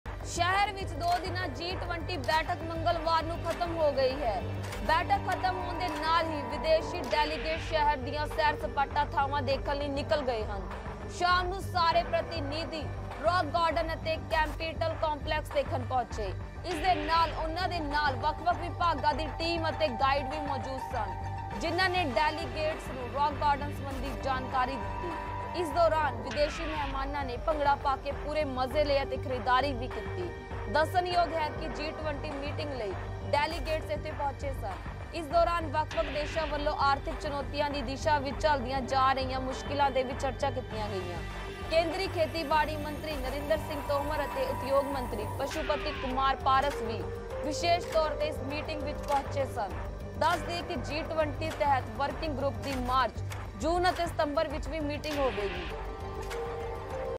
ਜੀ-20 ਬੈਠਕ ਮੰਗਲਵਾਰ ਨੂੰ ਖ਼ਤਮ ਹੋ ਗਈ। ਵਿਦੇਸ਼ੀ ਡੈਲੀਗੇਟਸ ਸ਼ਹਿਰ ਦੀਆਂ ਸੈਰ-ਸਪਾਟਾ ਥਾਵਾਂ ਦੇਖਣ ਲਈ ਨਿਕਲੇ।
ਸ਼ਾਮ ਨੂੰ ਸਾਰੇ ਪ੍ਰਤੀਨਿਧੀ ਰਾਕ ਗਾਰਡਨ ਅਤੇ ਕੈਪੀਟਲ ਕੰਪਲੈਕਸ ਦੇਖਣ ਪਹੁੰਚੇ। ਇਸ ਦੌਰਾਨ ਵਿਦੇਸ਼ੀ ਮਹਿਮਾਨਾਂ ਨੇ ਭੰਗੜਾ ਪਾ ਕੇ ਪੂਰੇ ਮਜ਼ੇ ਲਏ ਅਤੇ ਖ਼ਰੀਦਦਾਰੀ ਵੀ ਕੀਤੀ।